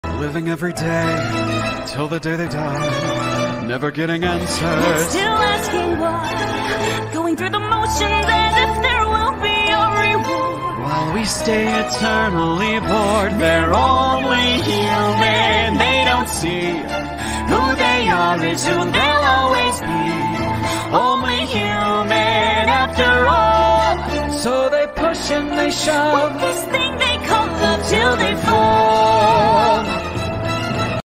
they're only Human.
an animatic between me and someone else's character!

the Blue one of Queen Radiance, and the Yellow one is Zircon!

they are cousins and he is the only royal family member who isn't trying to dethrone her!!!♥️♥️